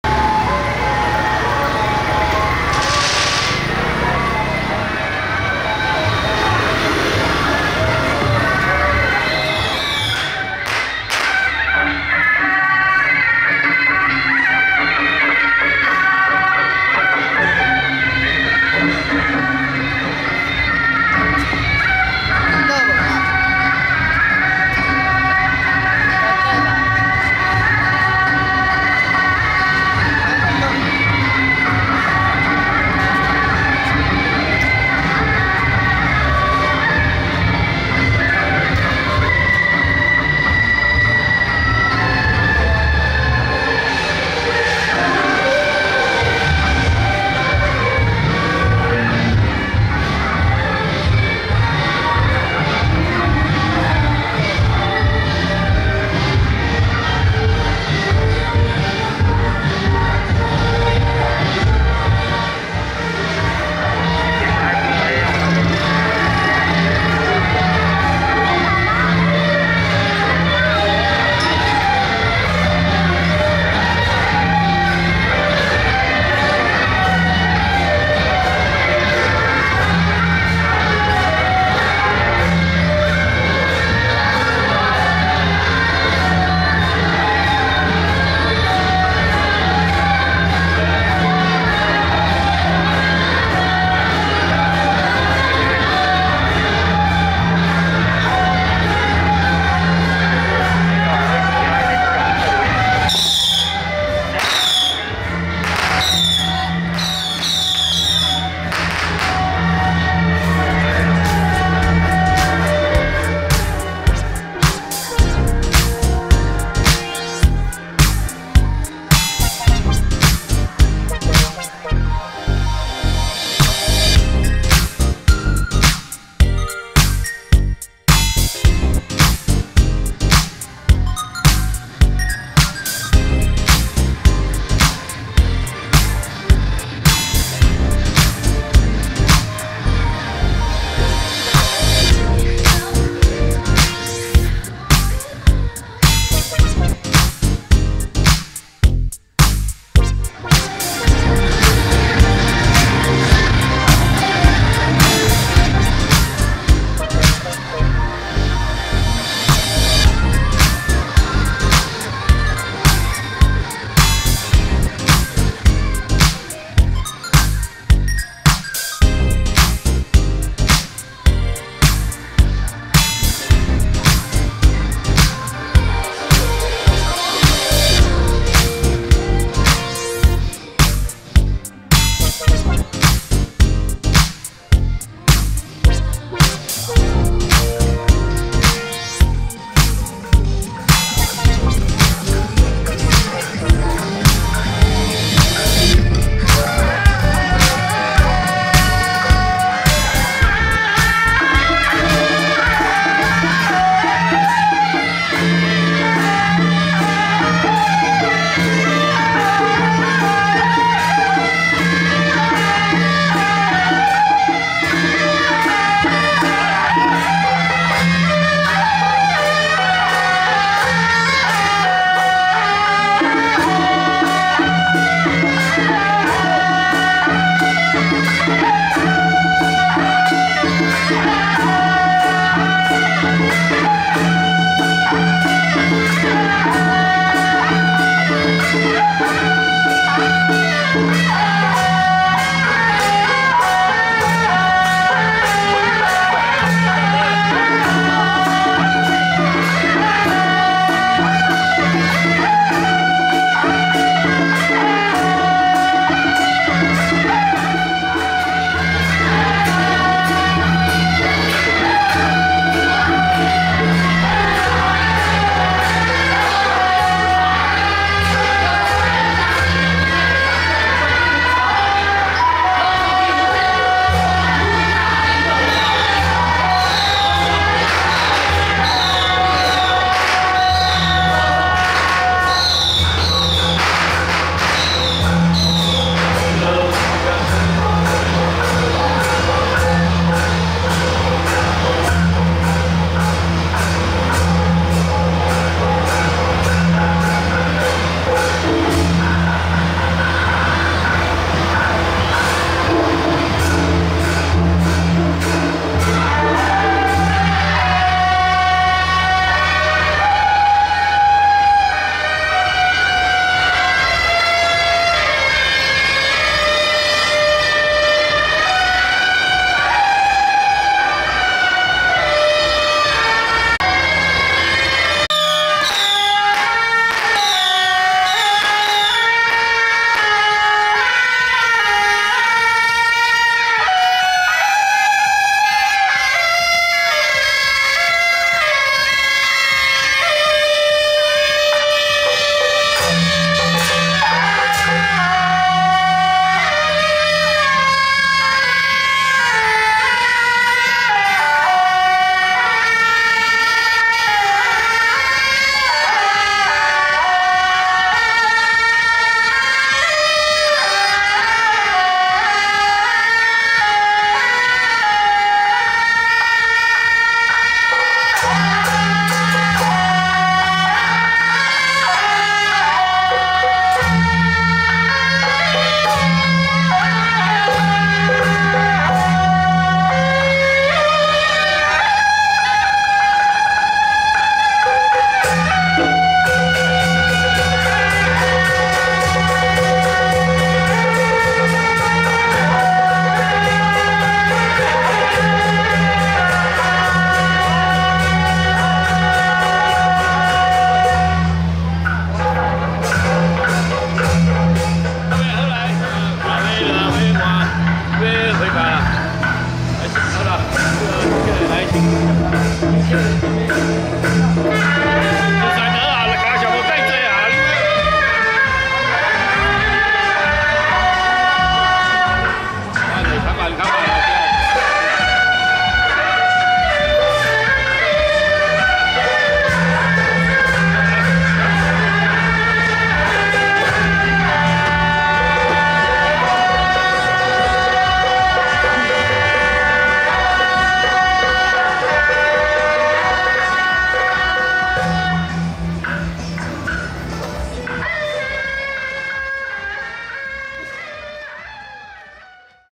《太子爺乩迎請聖母！歡喜拜池王爺》新園鹽埔仔 黃家天上聖母 謁祖進香回駕遶境~參拜境主 新興宮池府千歲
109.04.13屏東新園鹽埔仔 黃家天上聖母 前往嘉義朴子配天宮謁祖進香回駕遶境